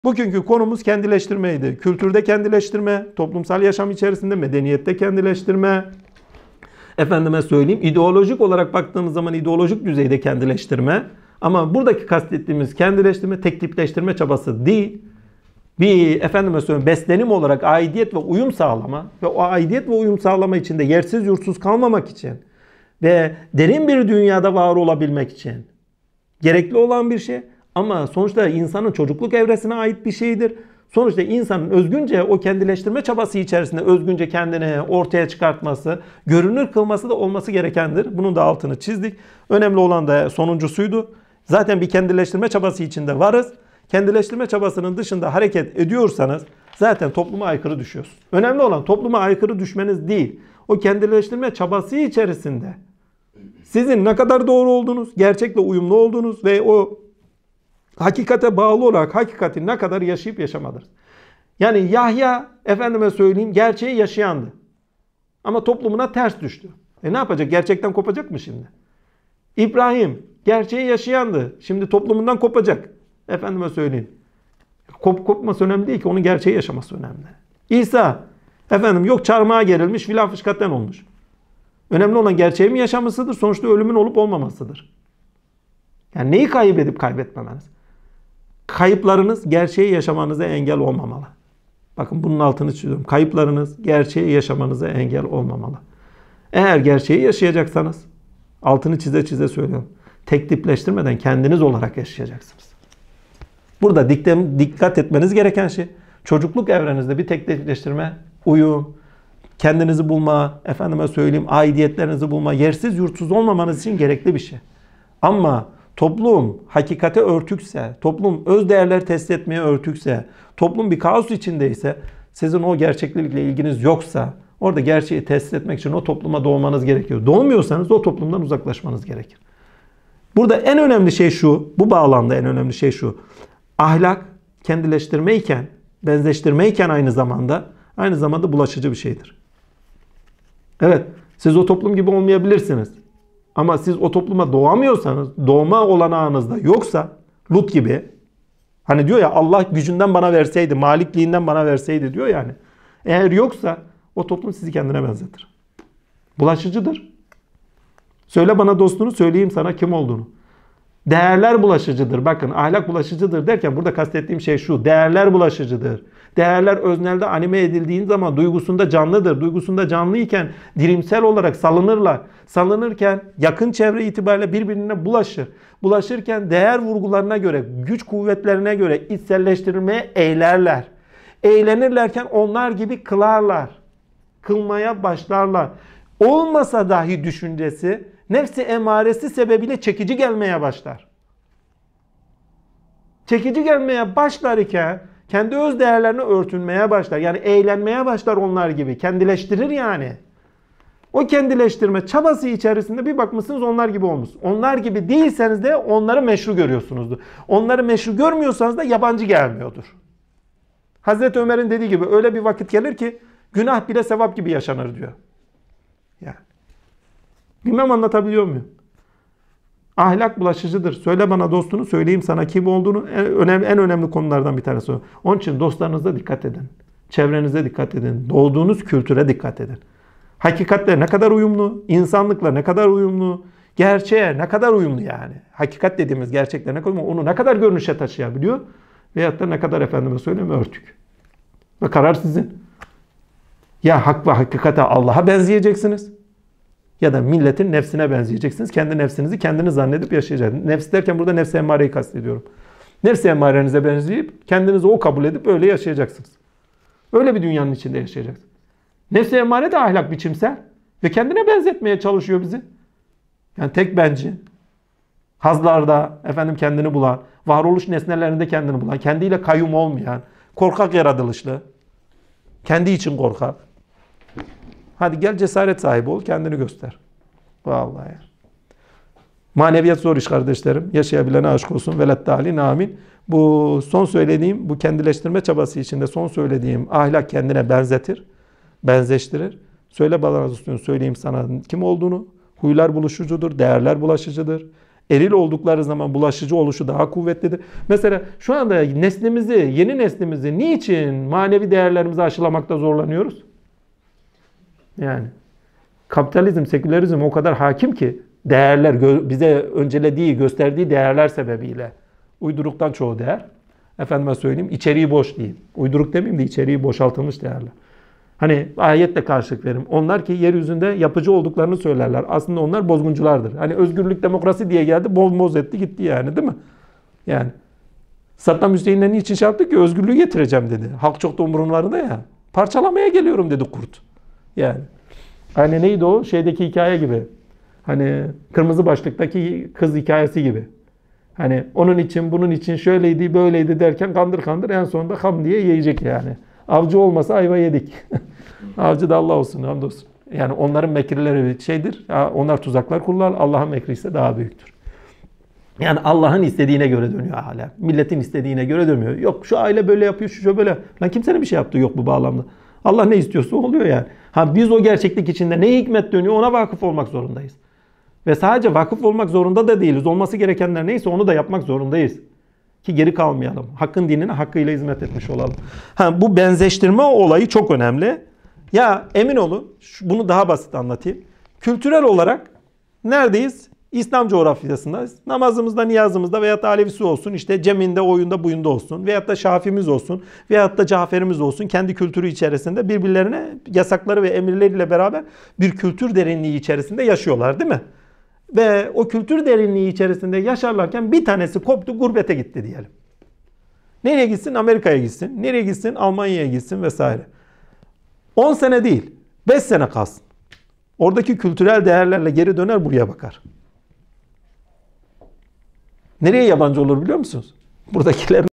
Ahlâk'ın Bulaşıcı Olması
Ahlâk ve huylar bulaşıcıdır. Konuyu daha iyi anlamak için lütfen aşağıdaki bağlantıyı tıklayarak dersin tamamını izleyiniz